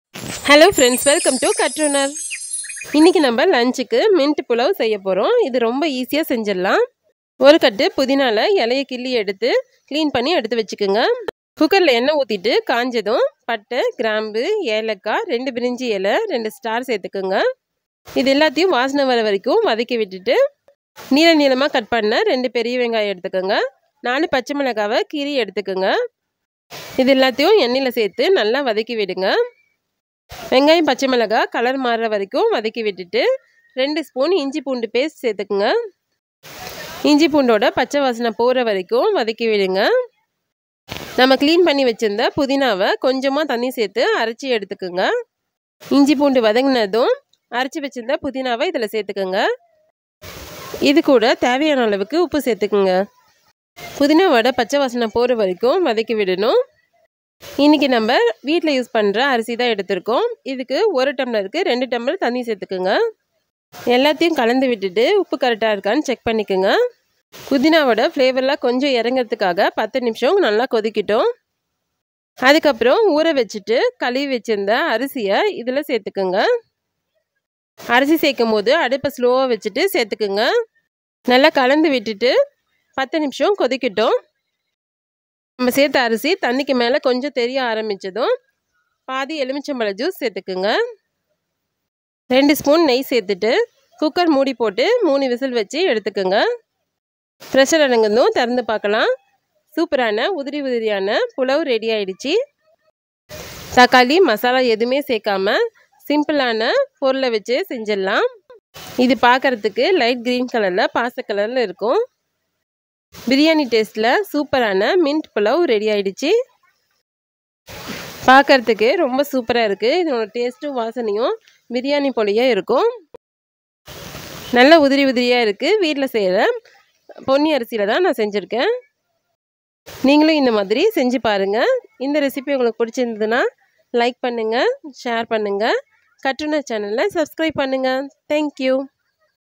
Pudina Pulao Without Tomato| Easy lunch box recipe|Mint Biryani without vegetable
lunchboxrecipe lunch easylunchbox MINT PULAO RECIPE | NO TOMATO, NO CHOP | EASY PUDHINA PULAO IN A PRESSURE COOKER

Crystal Salt - 1 tbsp
Bay leaf - 1
Cinnamon - 1-inch piece
Cloves - 2
Cardamom - 2
Star anise - 1/2 piece
Oil - 2 tbsp
Mint leaves - a handful
Green chillies - 2 to 3
Ginger - 1/2-inch piece